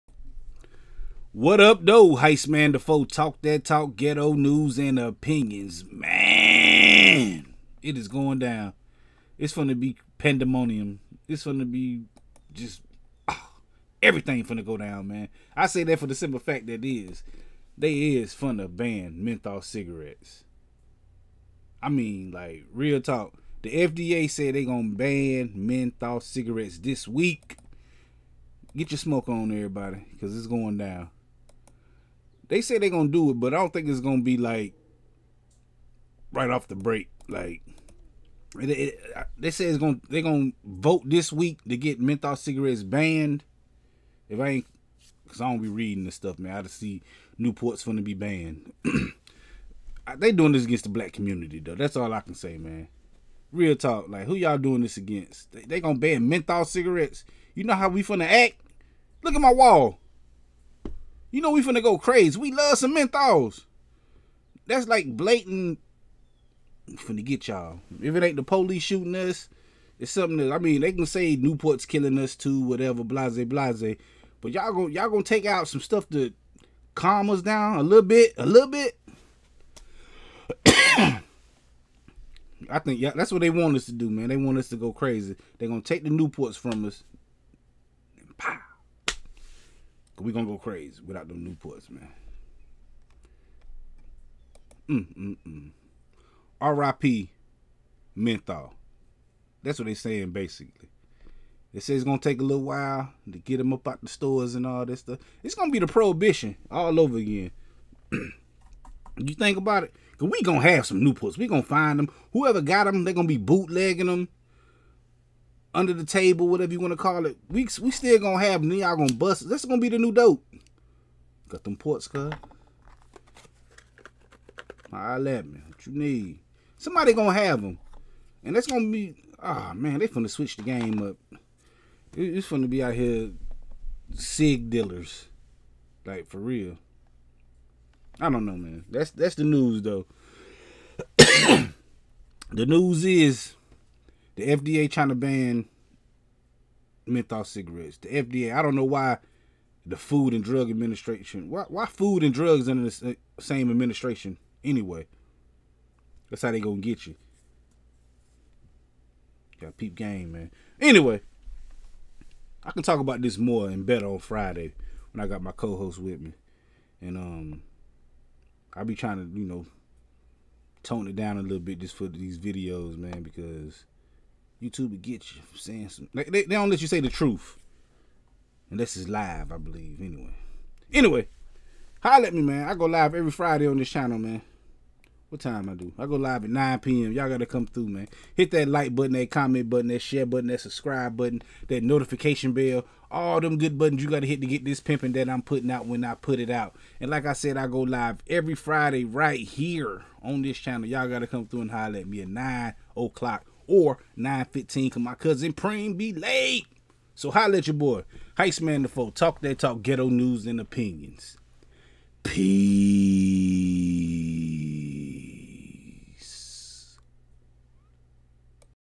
FDA Propose BAN on MENTHOL Cigarettes? Black People BEWARE!!
The FDA and Biden are proposing to ban Menthol cigarettes...They tryna get us.